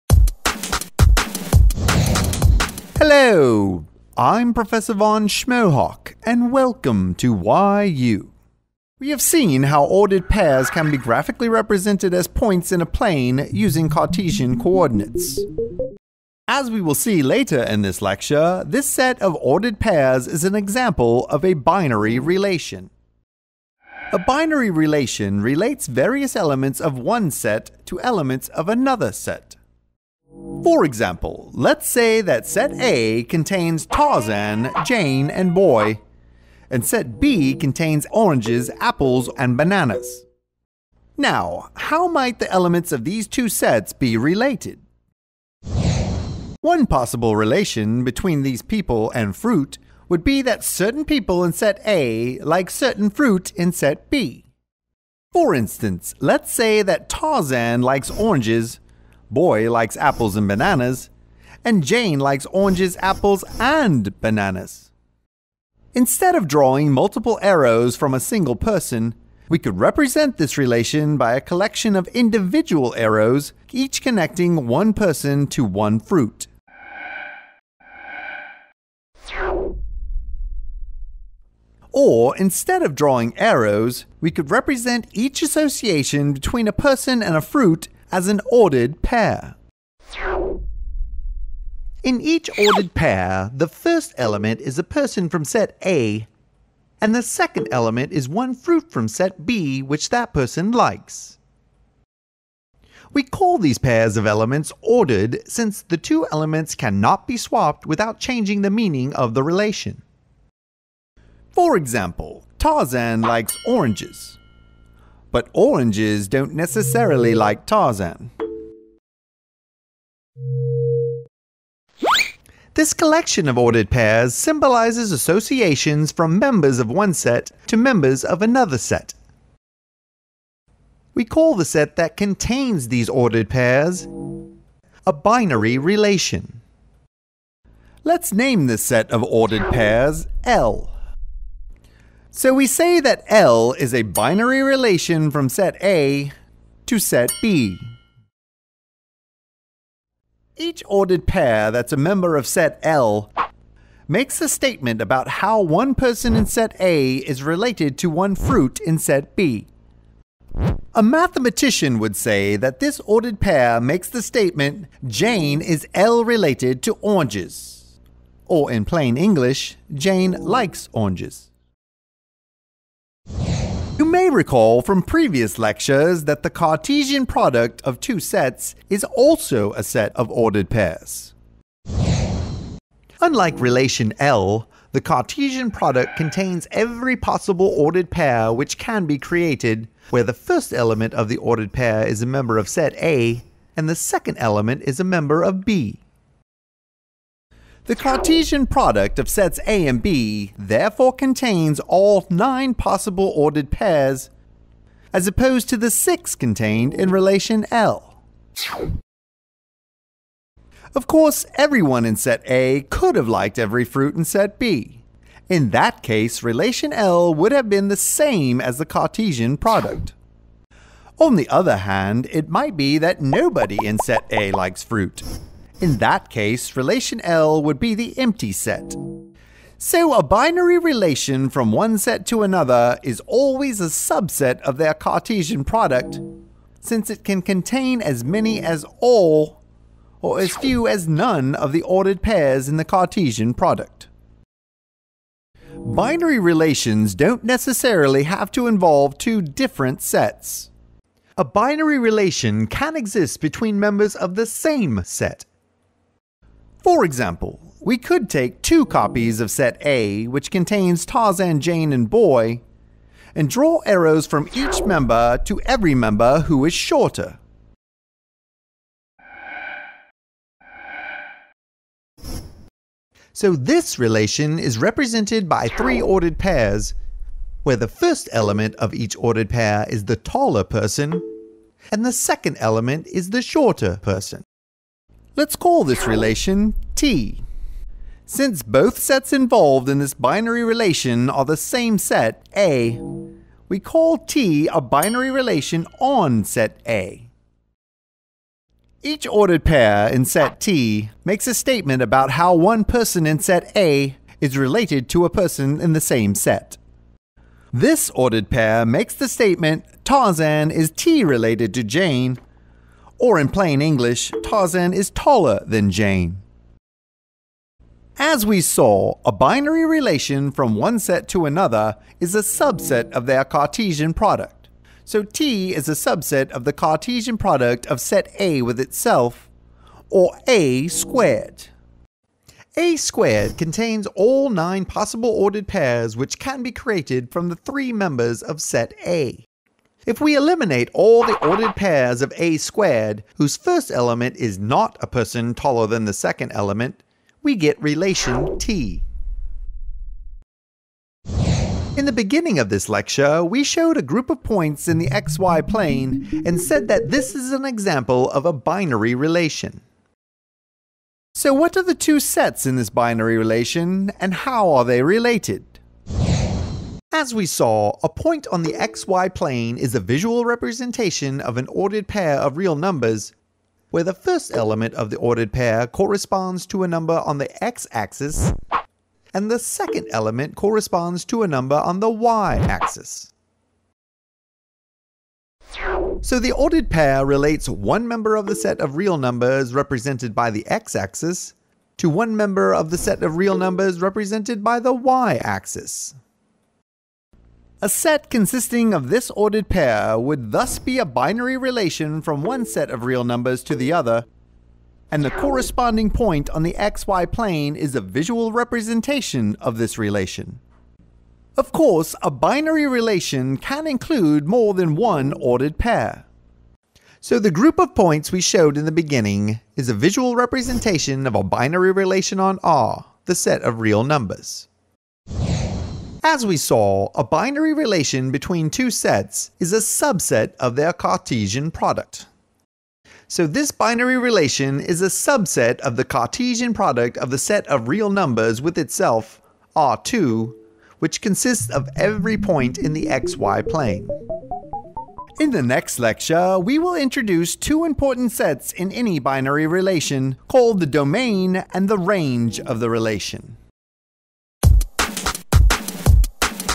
Algebra 12 - Binary Relations
Fundamental to Algebra is the concept of a binary relation. This concept is closely related to the concept of a function.